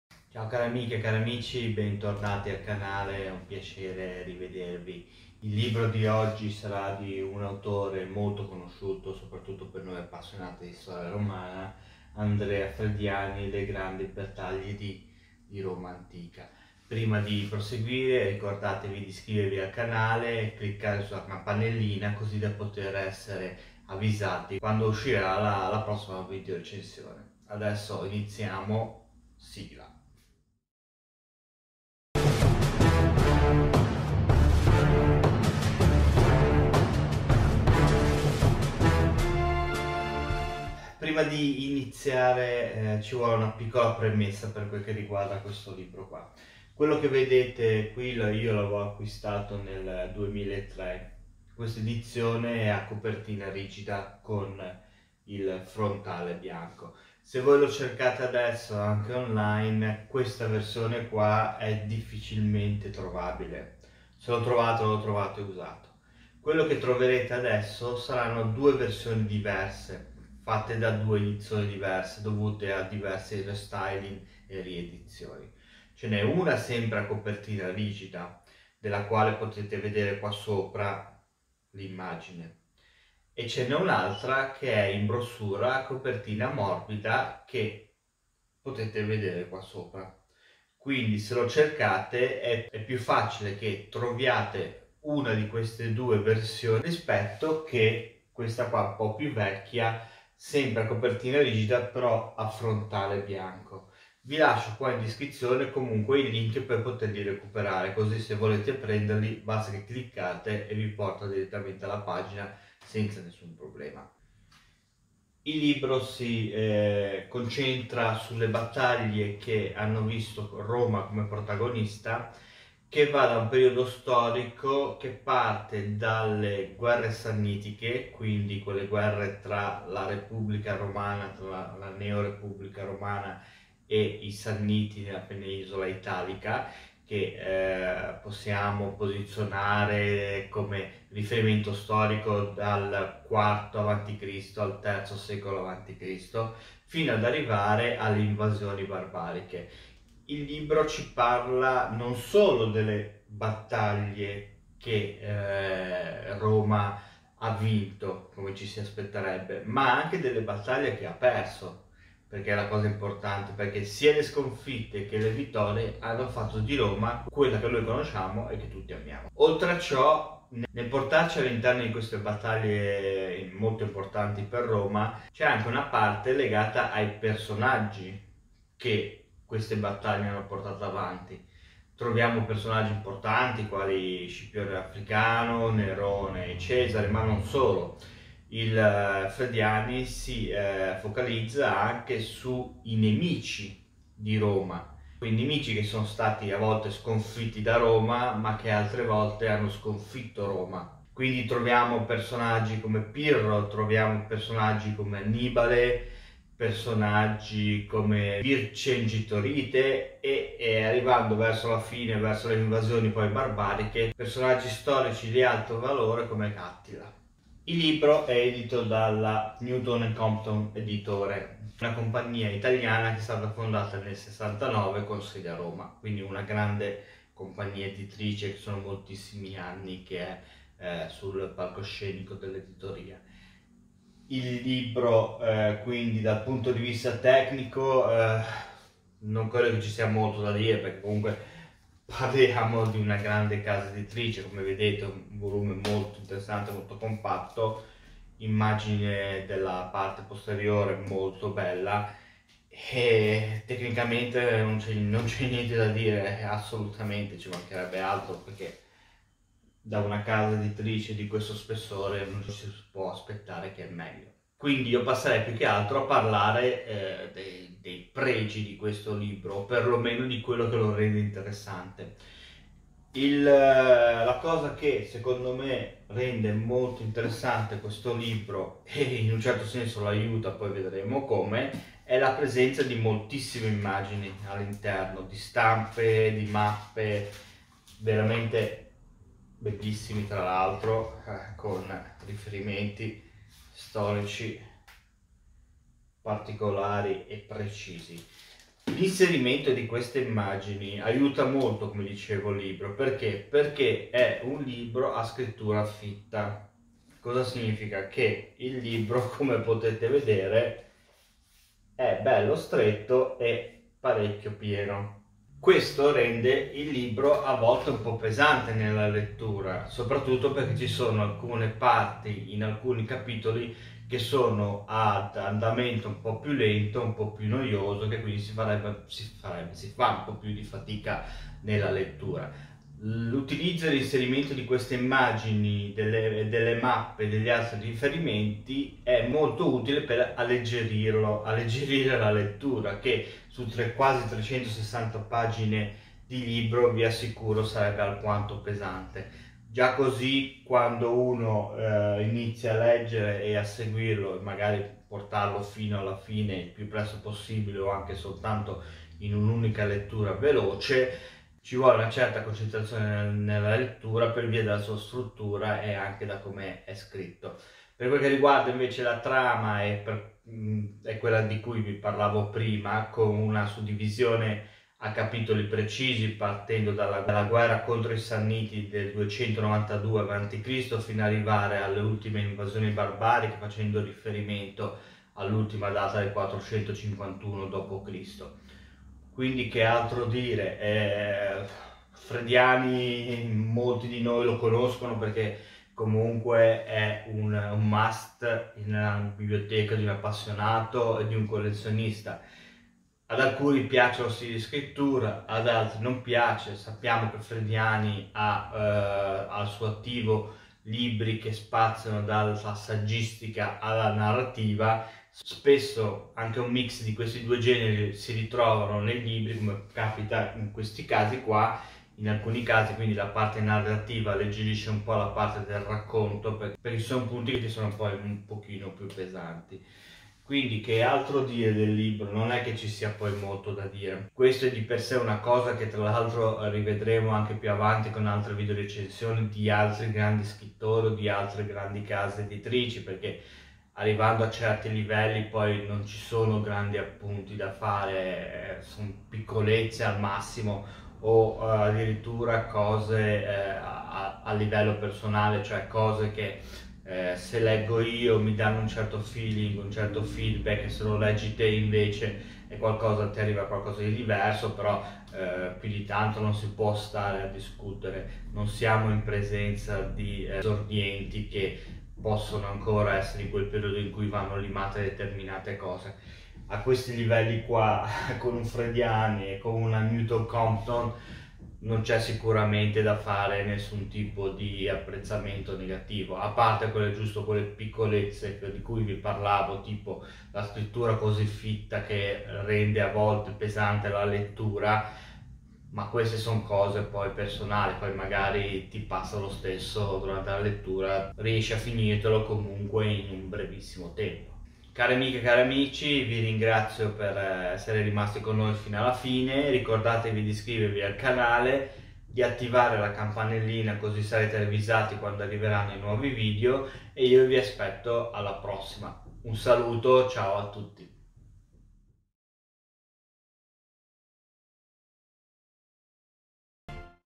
[Recensione] Le Grandi Battaglie di Roma Antica - Andrea Frediani
Un grande autore con una grande casa editrice un mix perfetto per un racconto/analisi sulle grandi battaglie di Roma antica, non solo quelle vinte con i suoi protagonisti storici come Scipione l'africano, cesare o Nerone, ma anche le sconfitte contro Annibale, Pirro, Vercingetorige e Attila.

- Le grandi battaglie di Roma antica
- Come Roma insegna
- L'incredibile storia degli imperatori romani
- L'inferno su Roma
- Le grandi guerre di Roma. L'età repubblicana